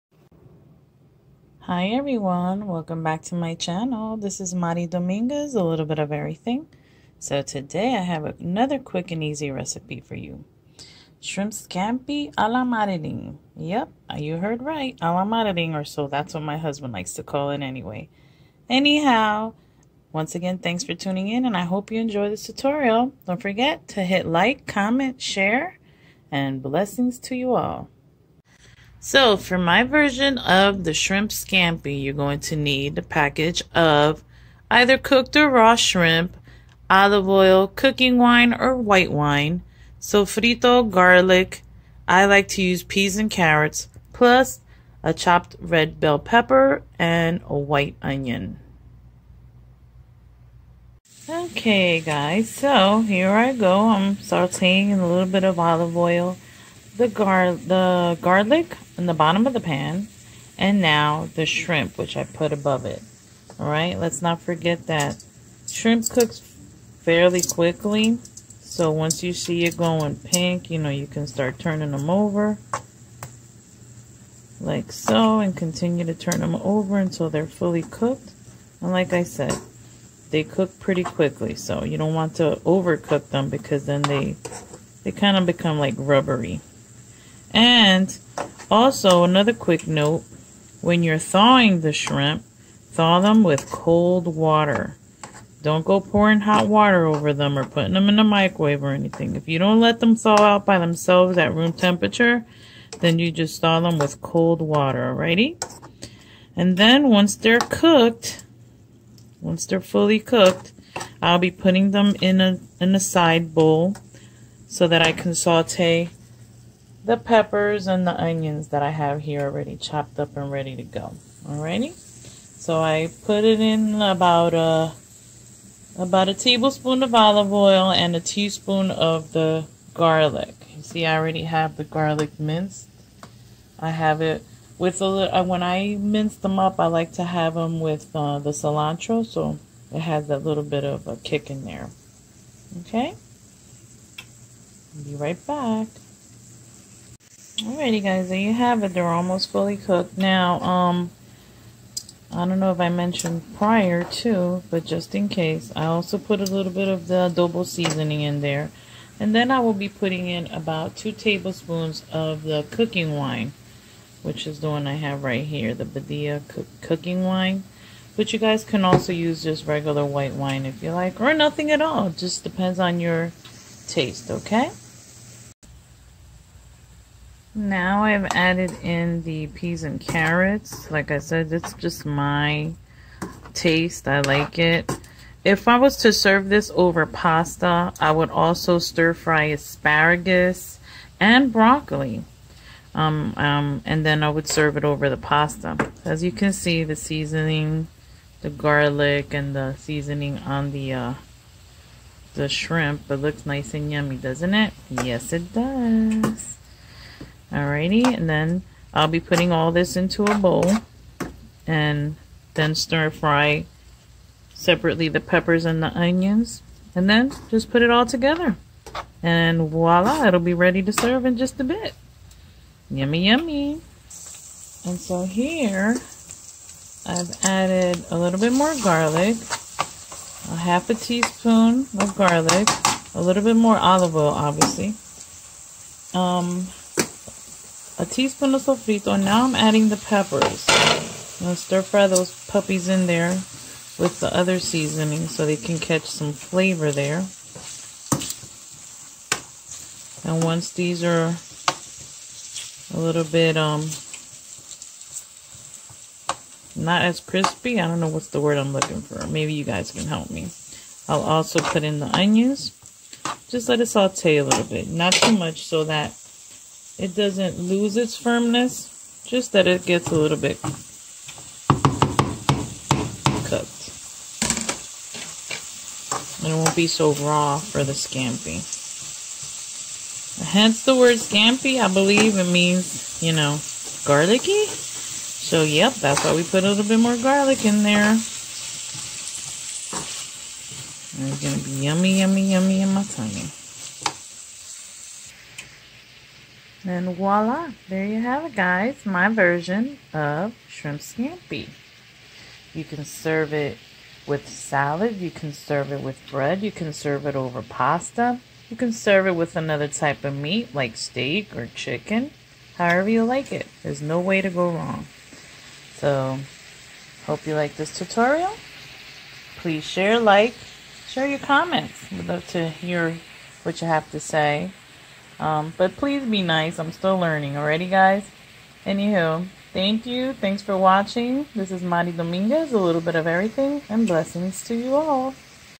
Easy Shrimp Scampi Recipe a'la Marilyn
Today I share with y'all another cooking tutorial.  For those that know me, know I love to cook & I wanted to share one of my fav dishes.  Once again, I greatly appreciate you all for bearing with me while I'm still on the learning process in the wonderful world of YouTube & vlogging!  Hope y'all enjoy this tutorial & would greatly appreciate your likes, shares and comments!

For this tutorial I used...
-1 12oz package of shrimp
-1 red bell pepper
-1/2 white onion
-1 1/2 teaspoons of minced garlic
-1 teaspoon sofrito [optional]
-1 cup peas & carrots [optional]
-2 tablespoons of white cooking wine
-3 tablespoons of olive oil
-Adobo Badia [or whatever seasoning you prefer to taste]